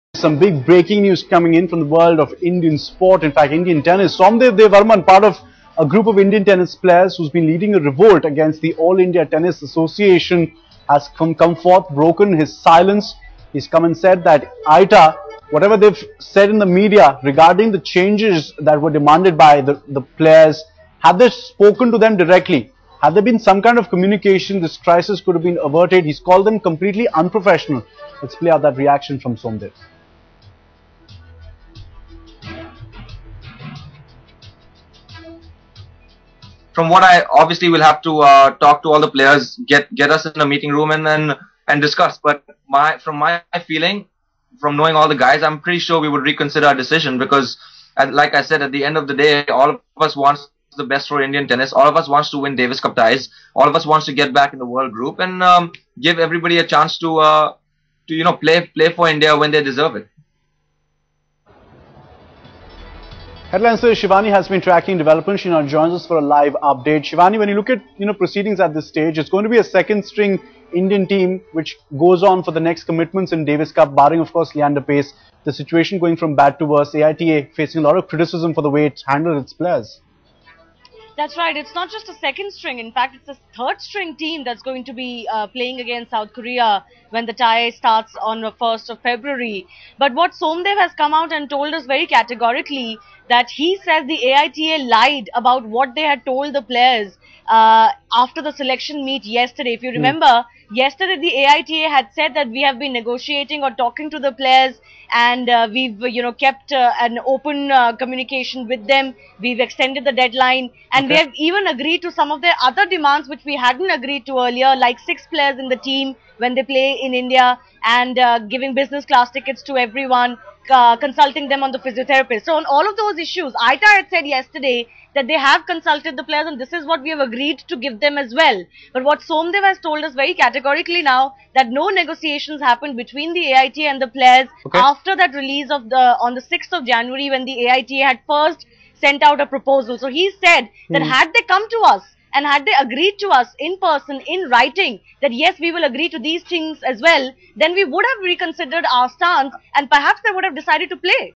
Somdev slams AITA for its unprofessional conduct
Indian tennis player Somdev Devvarman, who led a rebellion against the All India Tennis Association (AITA), said that the association has been acting in an unprofessional manner in handling their demands.